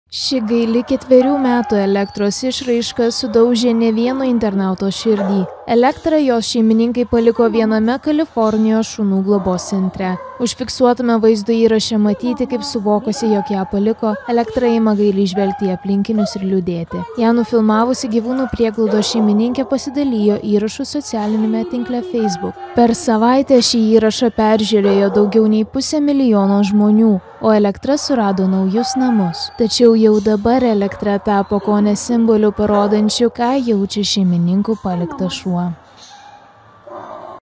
Nubrauksite ašarą: augintinė, sudaužiusi ne vieno internauto širdį
Ši gaili ketverių metų Elektros išraiška sudaužė ne vieno internauto širdį. Elektrą jos šeimininkai paliko viename Kalifornijos šunų globos centre. Užfiksuotame vaizdo įraše matyti, kaip suvokusi, jog ją paliko, Elektra ima gailiai žvelgti į aplinkinius ir liūdėti. Ją nufilmavusi gyvūnų prieglaudos šeimininkė pasidalijo įrašu socialiniame tinkle facebook. Per savaitę šį įrašą peržiūrėjo daugiau nei pusė milijono žmonių, o Elektra surado naujus namus.